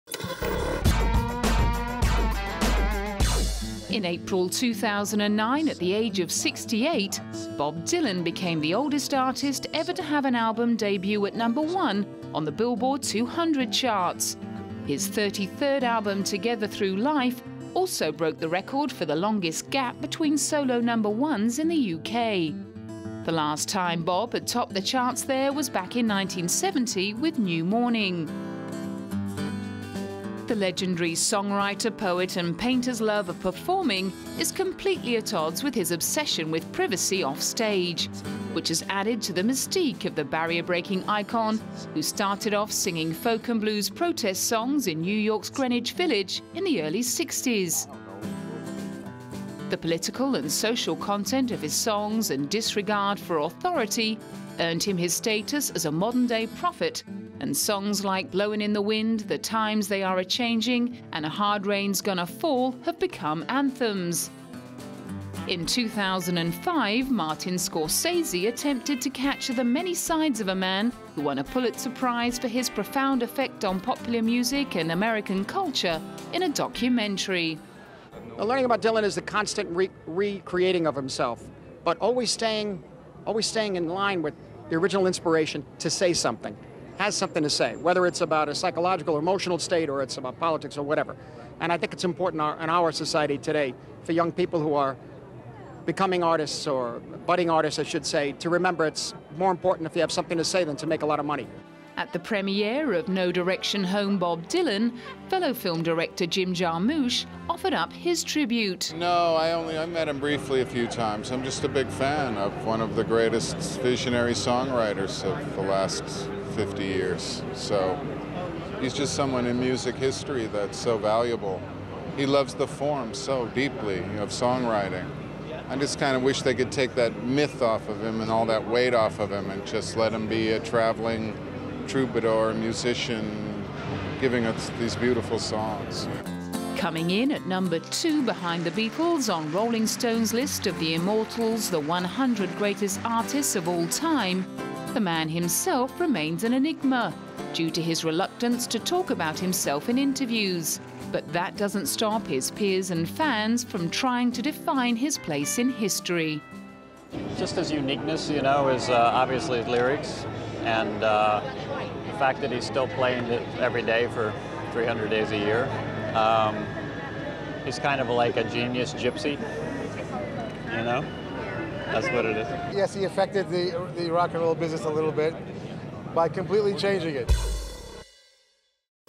The Greatest Bob Dylan | Popular Singer | Hit List | Rise To Fame Story | Upload 2016
Bob Dylan ( born Robert Allen Zimmerman, May 24, 1941) is an American singer-songwriter, artist and writer. He has been influential in popular music and culture for more than five decades.
Dylan's lyrics have incorporated various political, social, philosophical, and literary influences. They defied existing pop music conventions and appealed to the burgeoning counterculture. Initially inspired by the performances of Little Richard and the songwriting of Woody Guthrie, Robert Johnson, and Hank Williams, Dylan has amplified and personalized musical genres.

We are updating latest Buzz of Hollywood to keep you updated.